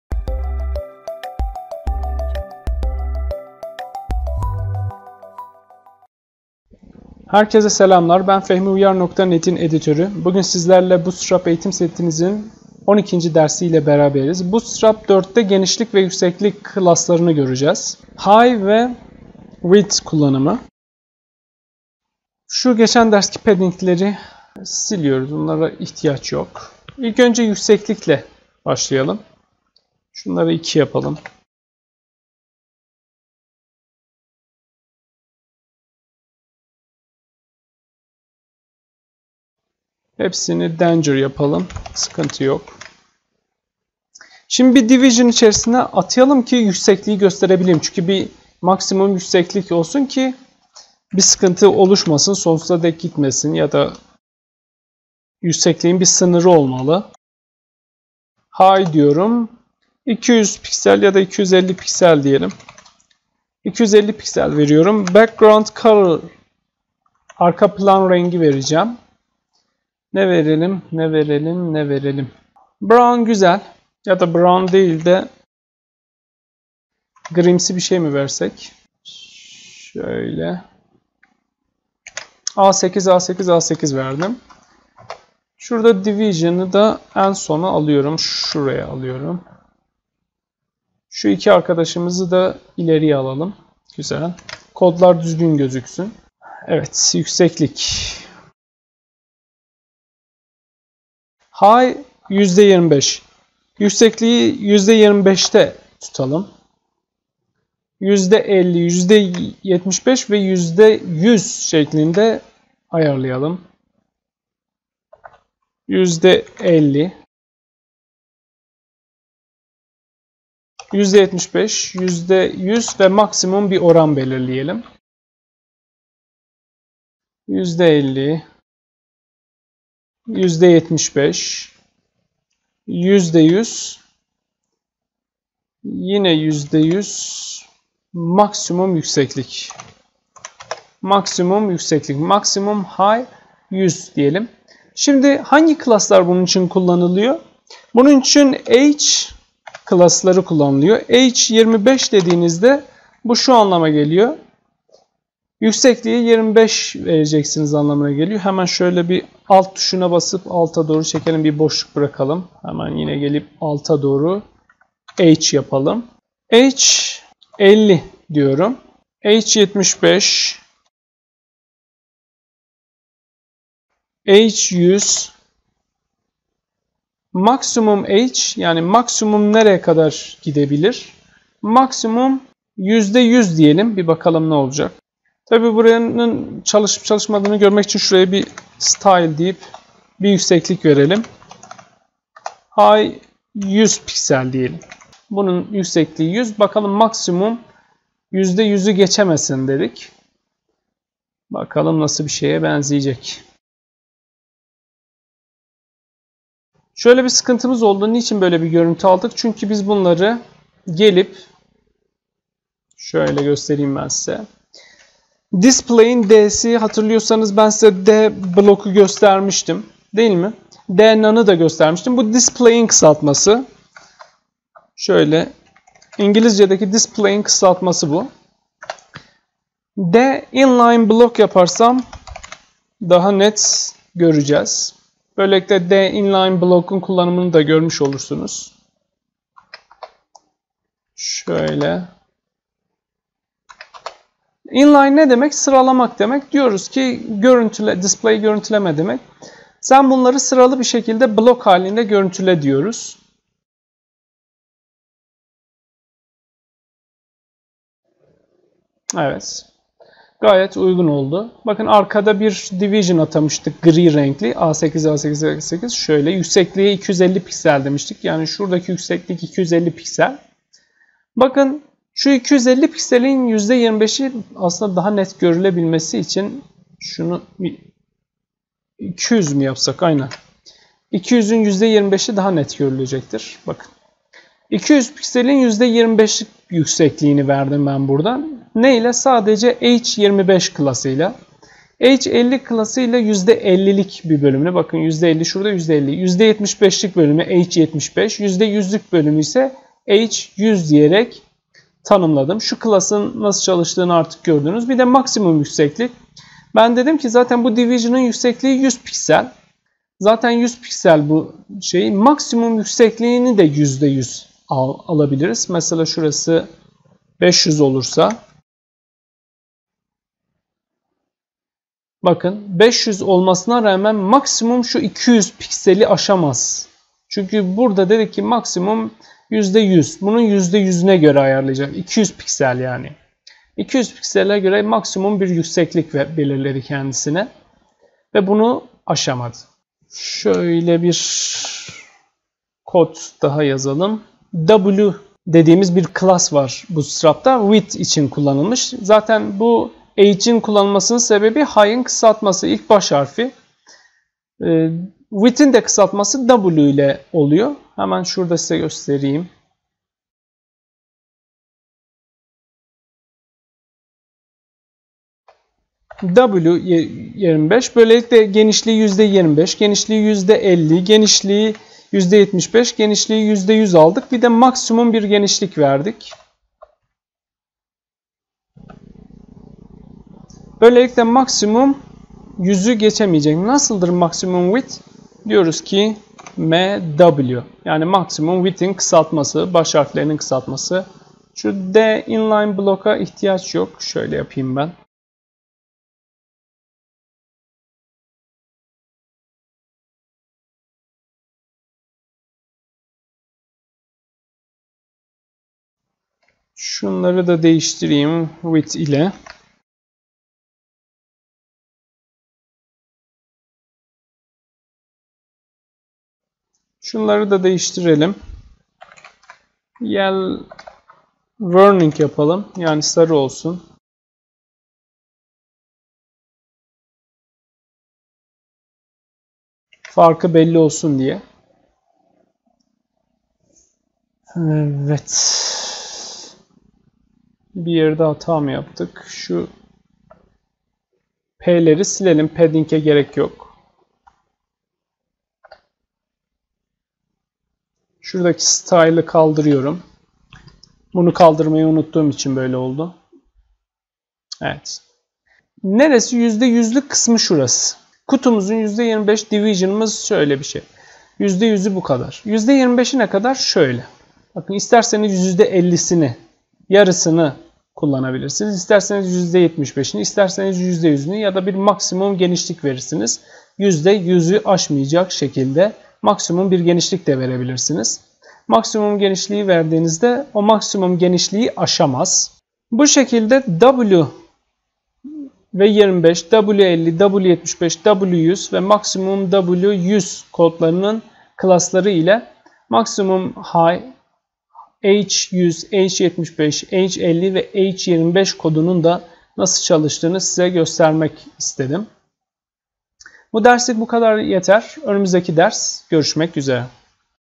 BS4 Ders 12 Yükseklik ve Genişlik Classları
Genişlik ve Yükseklik ile ilgili bazı classları anlattım.
Not! Reklam Engelleyici Kullanmayarak Emek Verenlere Destek Olun